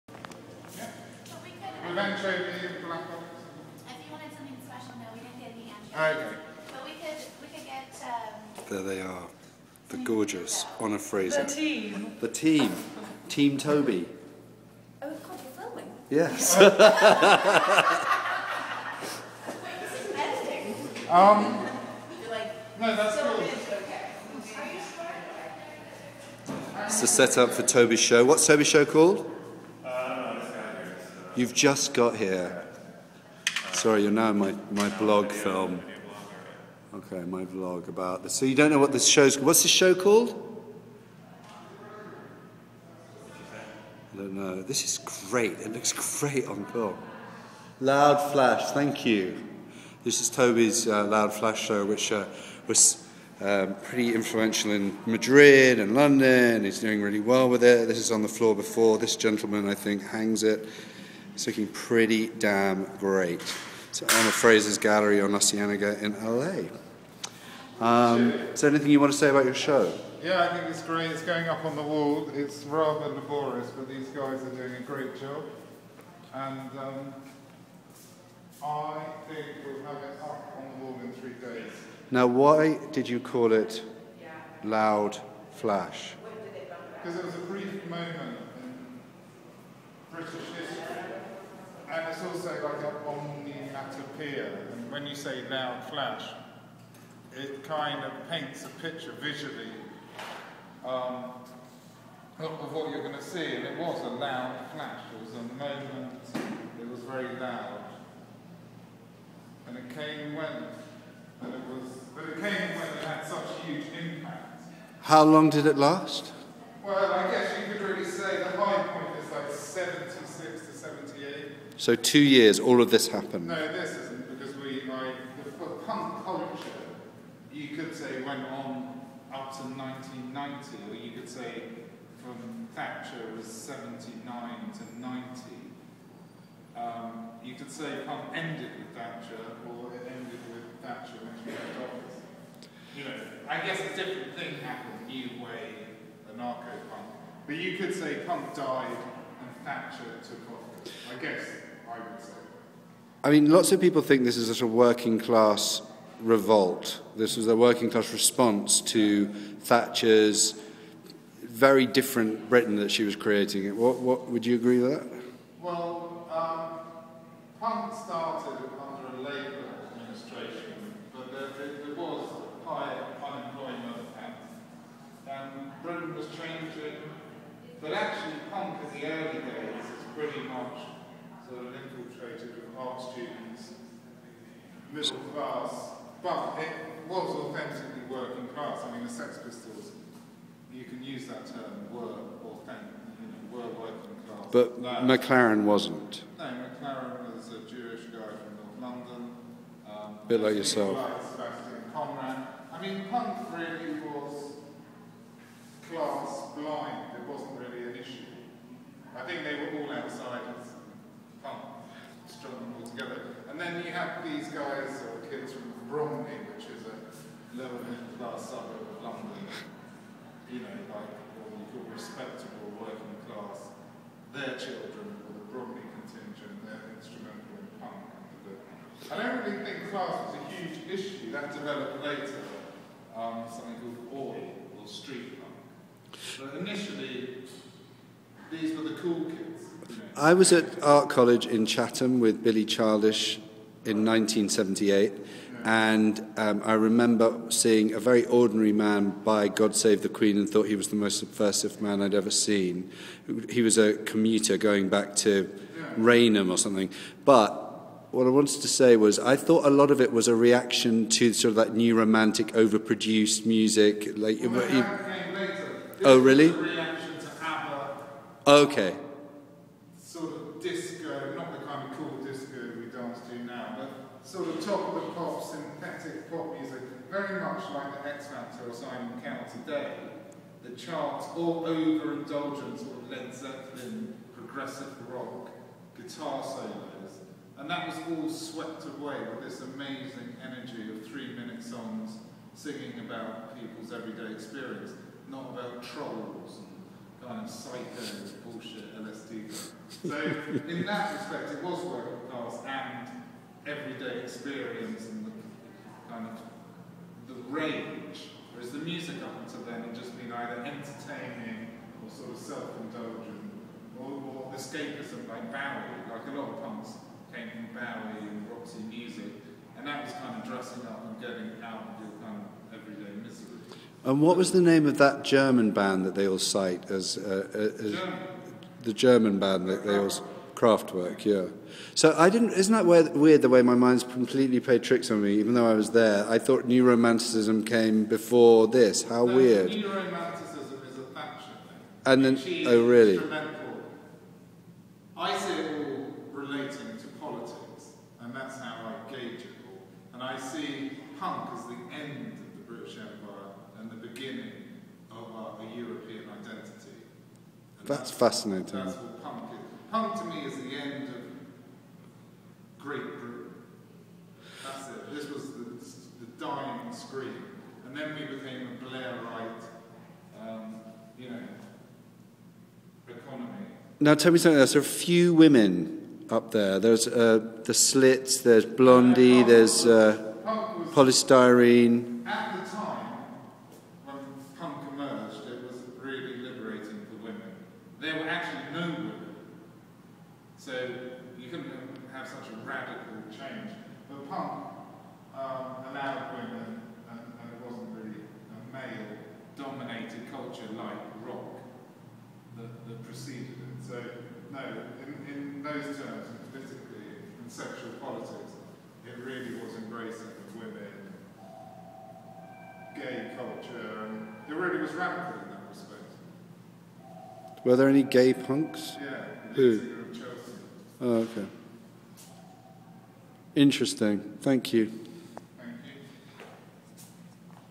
Punk on Paper Toby Mott The Honor Fraser Gallery
Toby Mott's show Loud Flash, the art of Punk opens at Honor Fraser's gallery in Los Angeles this Saturday.  I briefly interviewed him as the exhibition was being hung.

While the Sex Pistols and the Clash wreaked havoc on Britain's pop scene, their disciples were busy with glue and scissors, channelling punk's energy and DIY spirit into hundreds of posters, fanzines and sleeve art. Now an exhibition brings back these lost classics of the revolution